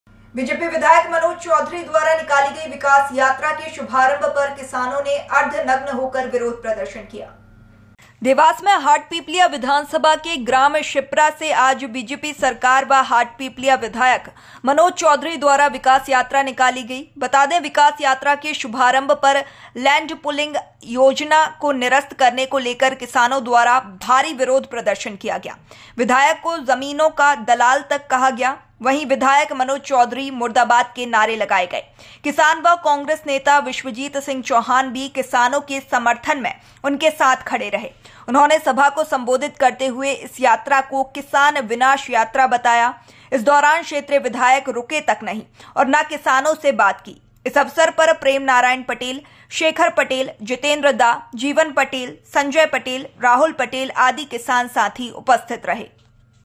देवास || किसानों ने अर्धनग्न होकर किया विरोध, योजना को निरस्त कराने की थी मांग
देवास में हाटपिपल्या विधानसभा के ग्राम शिप्रा से आज बीजेपी सरकार व हाटपिपल्या विधायक  मनोज चोधरी द्वारा  विकास यात्रा निकाली गई ।  विकास यात्रा के शुभारंभ पर लेंड पुलिंग योजना को निरस्त कराने को लेकर किसानो द्वारा भारी विरोध प्रदर्शन किया। विधायक को जमीनों का दलाल तक कहा गया व विधायक़ मनोज चौधरी मुर्दाबाद के नारे लगाया गए। किसान व कांग्रेस नेता विश्वजीत सिंह चौहान भी किसानो के समर्थन में उनके साथ खड़े रहे । उन्होने सभा को सम्बोधित करते हुए इस यात्रा को किसान विनाश यात्रा बताया। इस दौरान क्षेत्रिय विधायक रुके तक नहीं और ना किसानो से बात की। इस अवसर पर प्रेम नारायण पटेल, शेखर पटेल, जितेंद्र दा, जीवन पटेल, संजय पटेल, राहुल पटेल,  आदि किसान साथी उपस्थित हुए।

फेसबुक

इंस्टाग्राम

ट्विटर